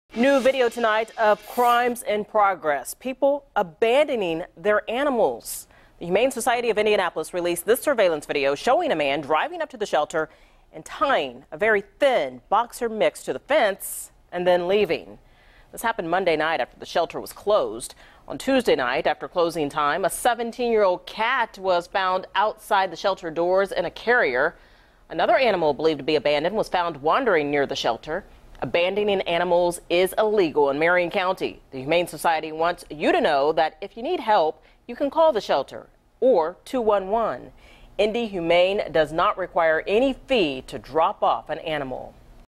Three pets abandoned outside IndyHumane this week in freezing temps
Three animals were found by IndyHumane workers this week after apparently being abandoned outside the Michigan Road shelter.